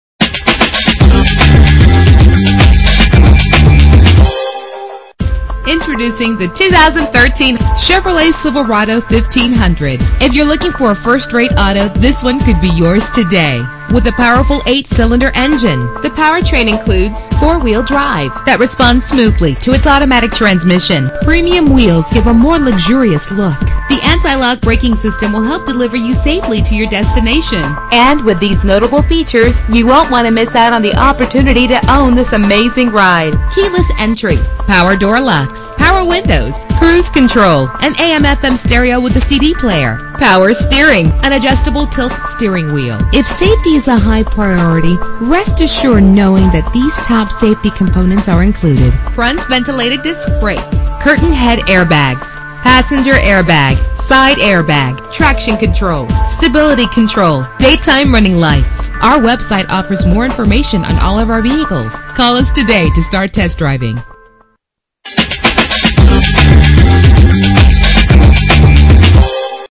2013 Chevrolet Silverado 1500 - CHESHIRE MA
Year: 2013
Make: Chevrolet
Model: Silverado 1500
Trim: WT
Engine: 5.3 liter 8 cylinder 16 valve
Transmission: Automatic
Color: Silver Ice Metallic
Mileage: 51
Address:
452 S State Rd